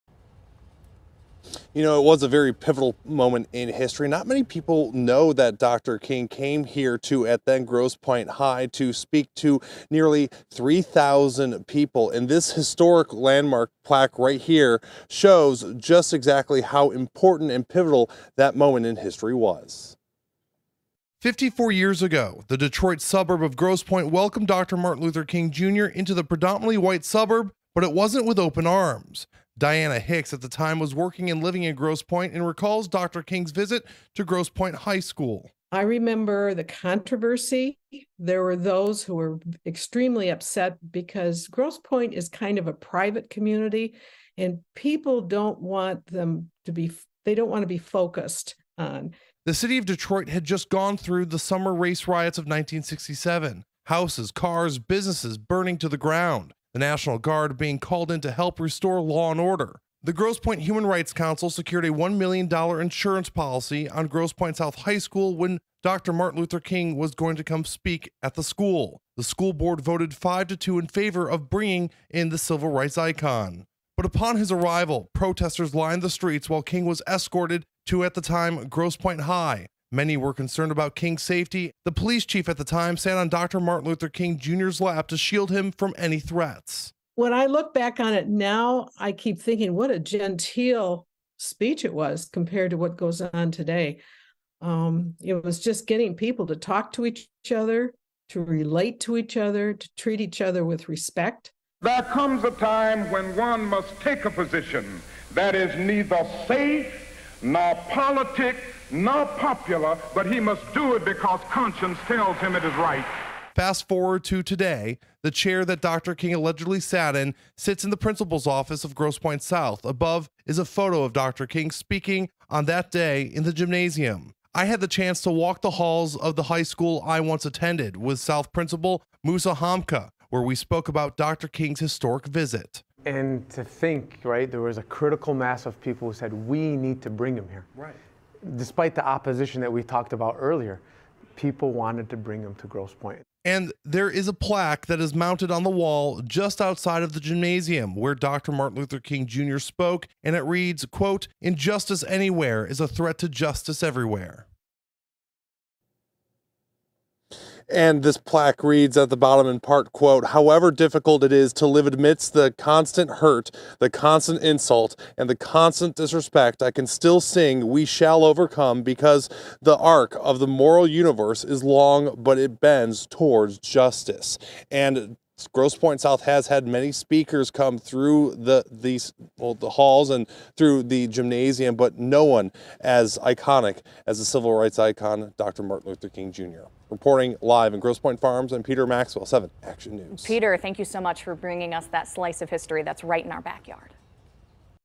Dr. Martin Luther King Jr.'s speech in Grosse Pointe
In front of Grosse Pointe South High School on Fisher Road sits a historic plaque marking the day Dr. Martin Luther King Jr. spoke at the school.

Nearly 3000 people packed the school's gymnasium as King delivered his  "The Other America" speech.

​Diana Hicks was working and living in Grosse Pointe at the time and recalls Dr. King's visit. She said the Alabama preacher wasn't welcomed into the predominantly white suburb with completely open arms.   ​"I remember the controversy. There [were] those who were extremely upset because Grosse Pointe is kind of a private community and people don't want to be focused on," she said.  ​

The city of Detroit had just gone through the Summer Race Riots of 1967. Houses, cars, and businesses were burning to the ground and the National Guard was called in to help restore law and order. ​  Around the same time, the Grosse Pointe Human Rights Council voted 5-2 in favor of bringing Dr. Martin Luther King Jr. to Grosse Point. The school board then secured a $1 million insurance policy on Grosse Pointe South High School to have the speech held there.